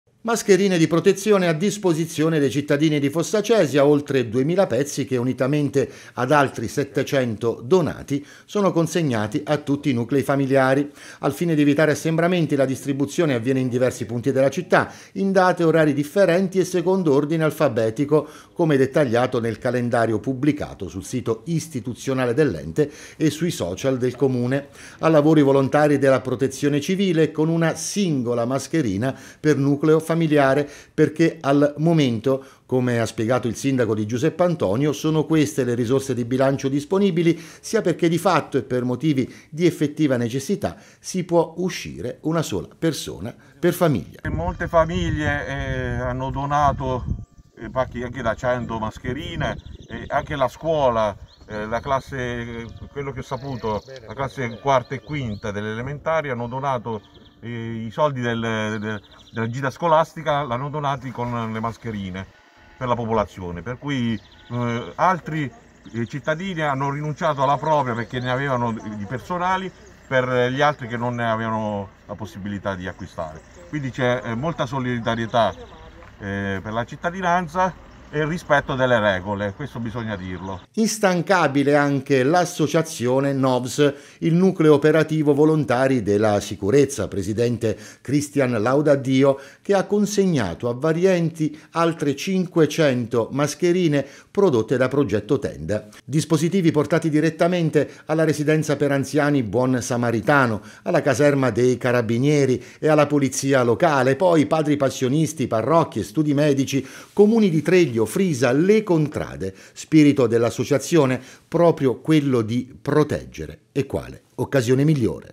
Mascherine arrivano a Fossacesia
Fossacesia: protezione civile e volontari stanno distribuendo migliaia di mascherine a cittadini ed enti. Previsto un dispositivo per ogni nucleo familiare